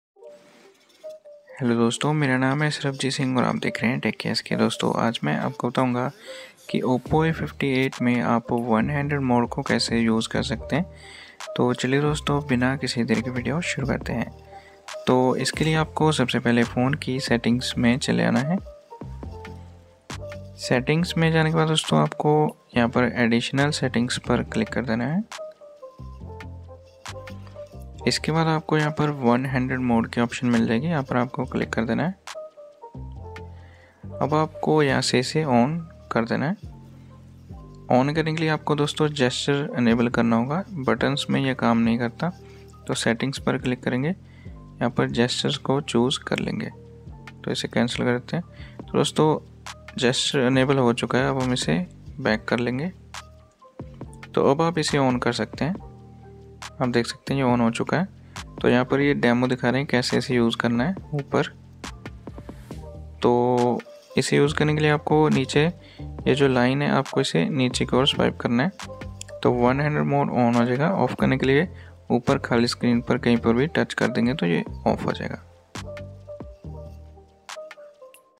How to enable one-handed mode in Oppo A58 || Oppo A58 me one-handed mode kaise enable kare
Hello dosto, aaj maine is video me bataya hai ki Oppo A58 me one-handed mode kaise enable kare, dosto Oppo A58 me one-handed mode enable karna bahut aasan hai. Agar aap bhi janna chahate hain ki Oppo A58 me one-handed mode kaise enable kare to is video ko pura dekhiyega, thanks friends.

Hello friends, in this video, I've explained how to enable One-Handed Mode on Oppo A58. Enabling One-Handed Mode on Oppo A58 is very easy. If you want to know how to enable One-Handed Mode on Oppo A58, be sure to watch this entire video. Thanks, friends.